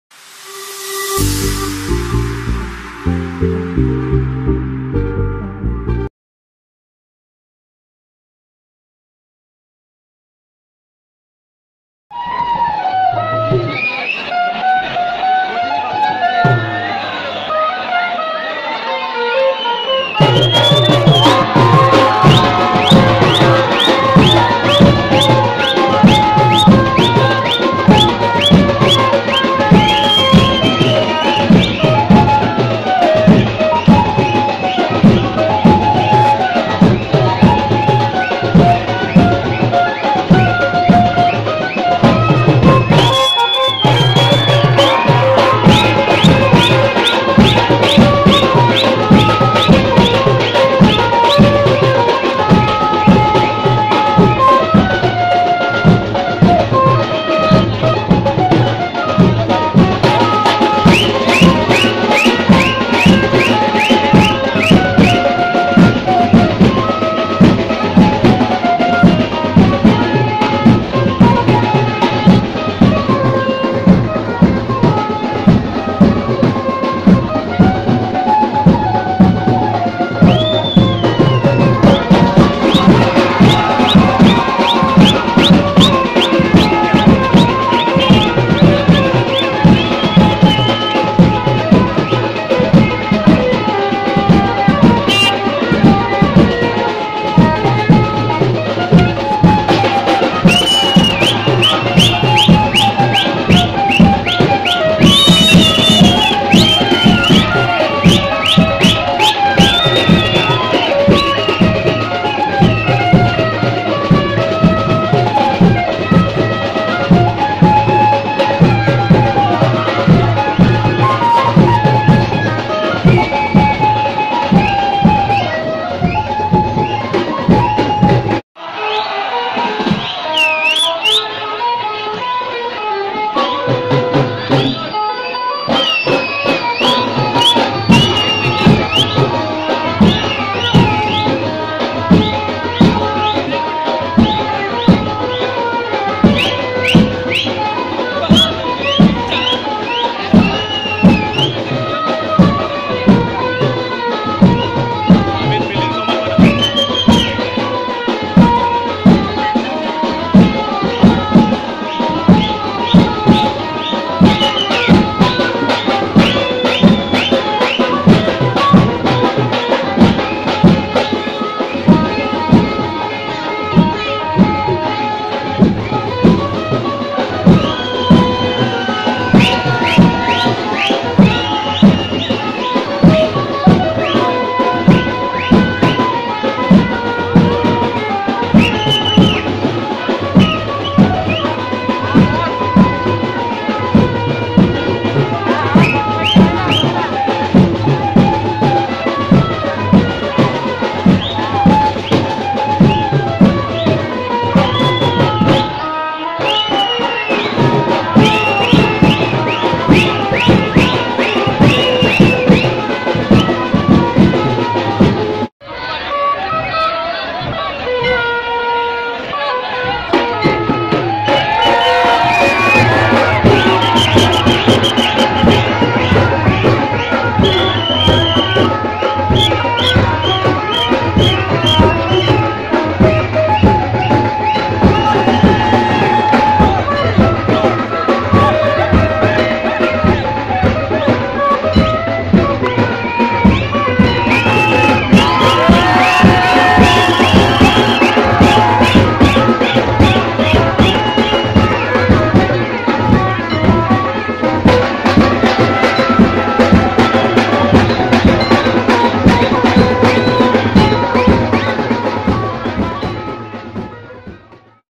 Girgaoncha Ladka Milind | Girgaon Cha Raja
Girgaon Cha Ladka Milind Jangam

Swami Samarth Banjo Group play in Girgaon Cha Raja 2017